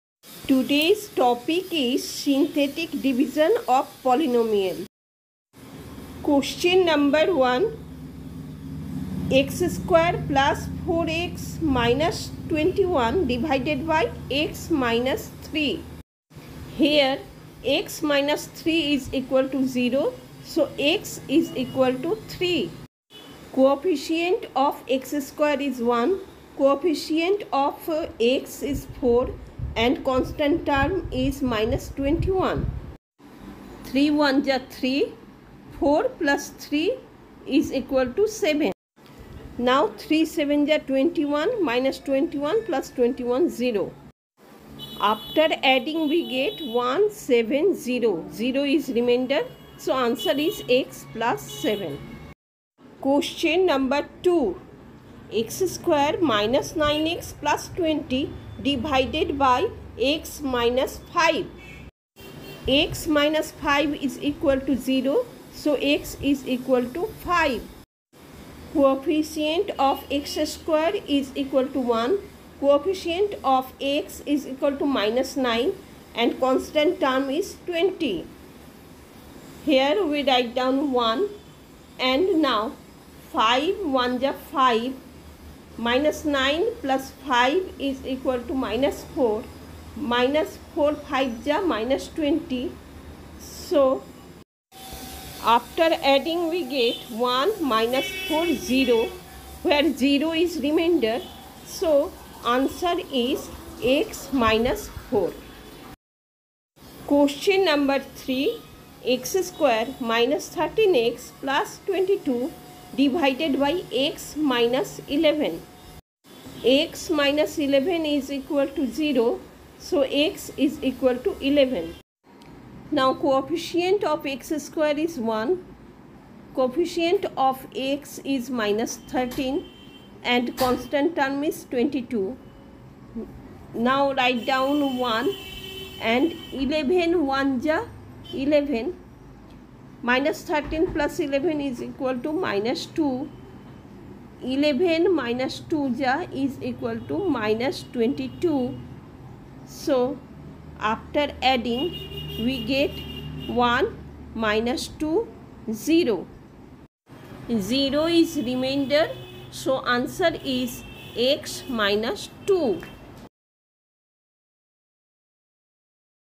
Synthetic division of polynomials
Synthetic division of polynomials ‎@DeeptiMukherjee1
Here, I have explained the synthetic division method of polynomials. We can also divide these polynomials using long division. This is a shortcut method of division. Keep watching.
Thanks.
Deepti Mukherjee.